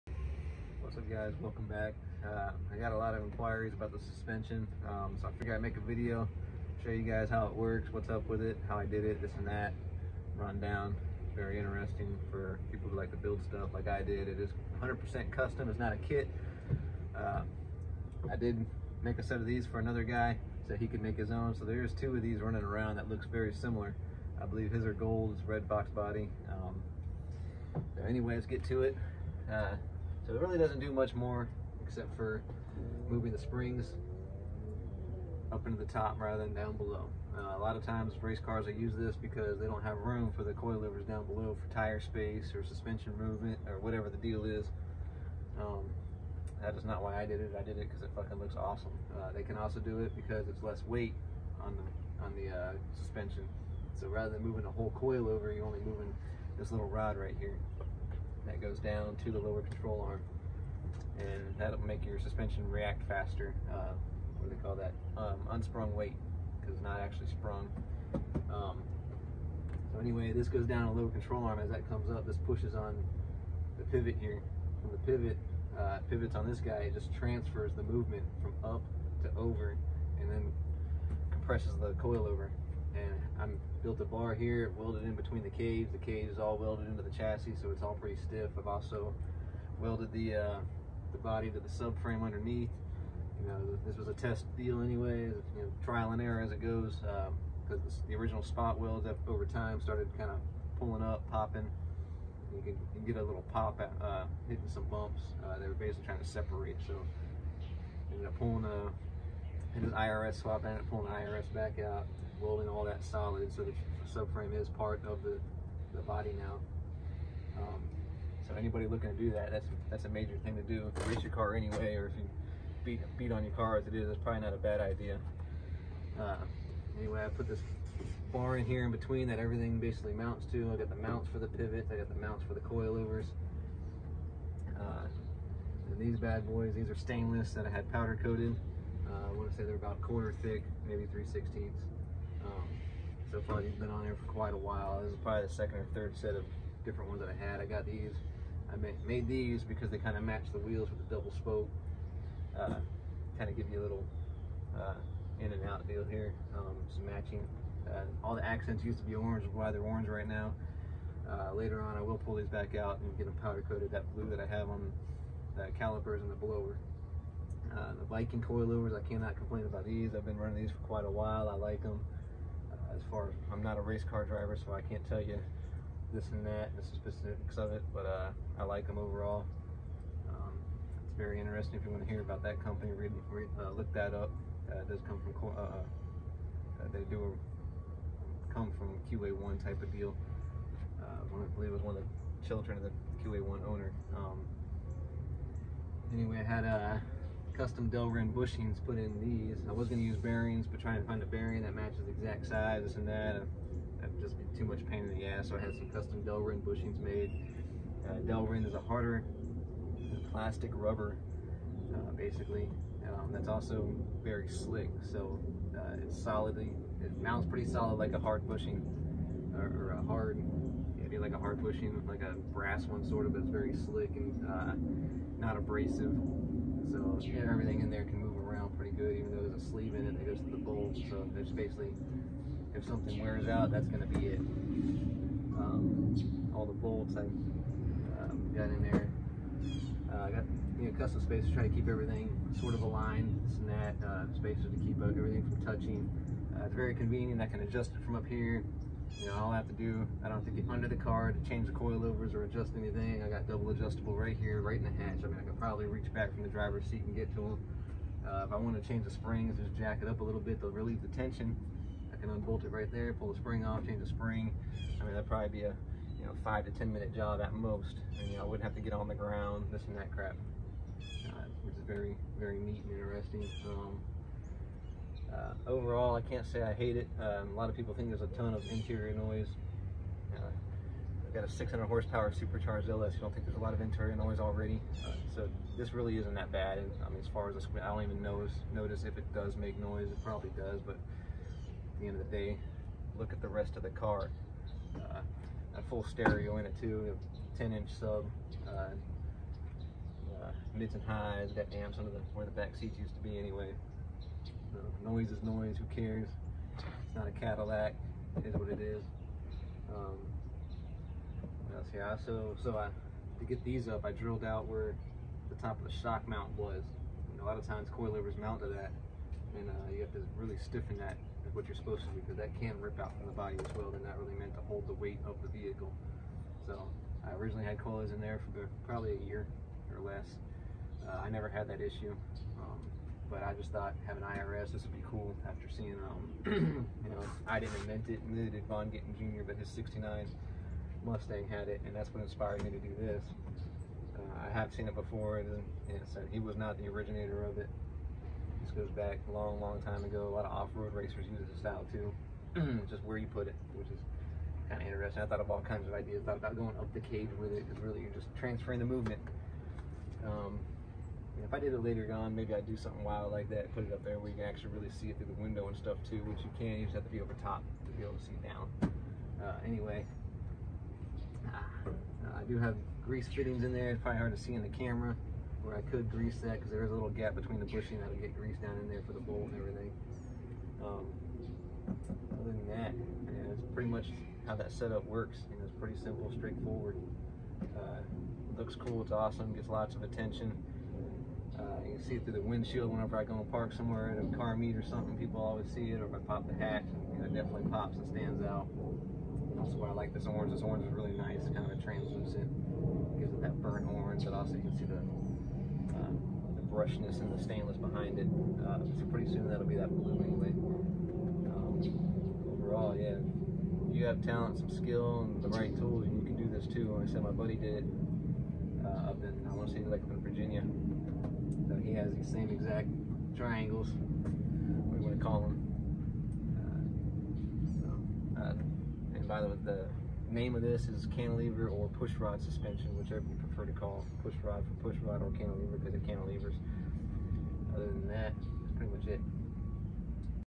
Fox body cantilever suspension explanation
Explaining how the cantilever, or pushrod, suspension in the mustang works. It is custom built, not a kit. If you are looking to build your own, I hope this helps get you steered in the right direction.

Fox body mustang cantilever suspension pushrod coilovers independent rear suspension irs